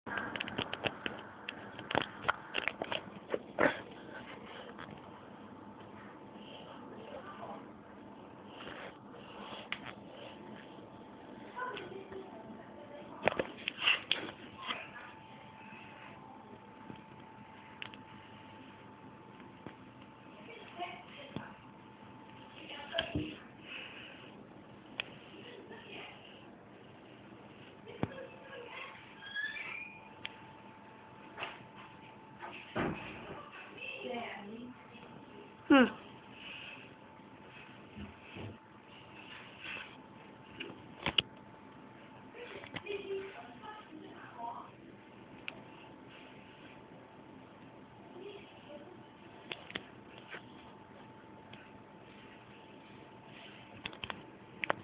Cool Spinning Eraser - 38 Secs
Hey guys, i have created a spinning eraser that can spin for 38 secs, thats my current record, for those who think that they can beat me. Just post a responce vid, in time i'll try to improve my record.